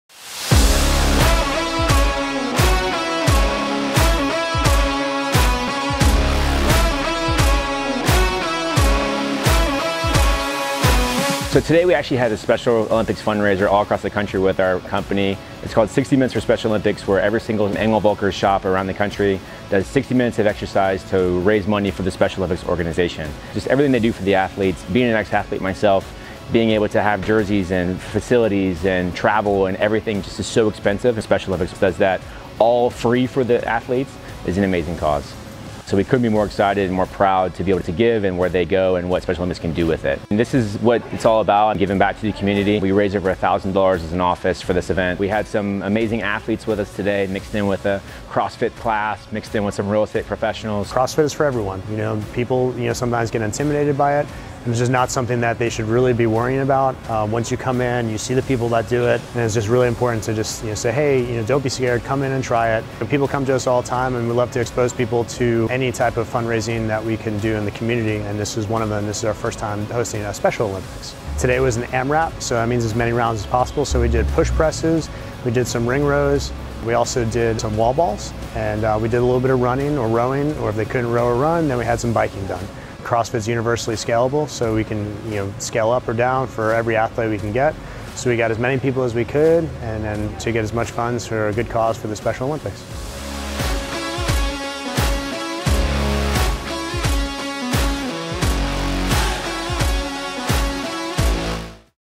60 Minute for Special Olympics 2022 with E&V Jax!
We are excited to share that our network has exceeded our 60 Minutes for Special Olympics fundraising goal, raising a record-breaking $102,399.99 over the past weeks. Locally, our shop raised over $1,200!